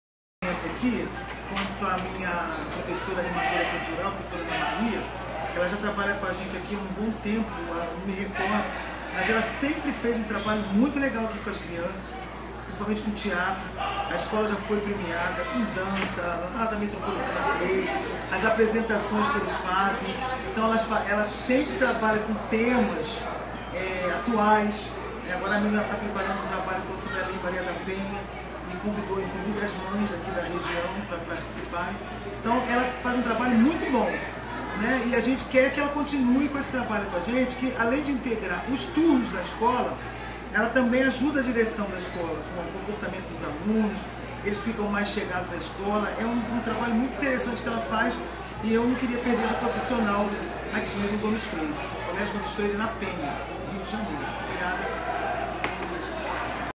C.E. GOMES FREIRE DE ANDRADE
Nossa Diretora Geral Profª Elomar Moura Santos falando sobre a importância das Atividades Culturais por mim desenvolvidas no Gomes Freire - Penha - Rio de Janeiro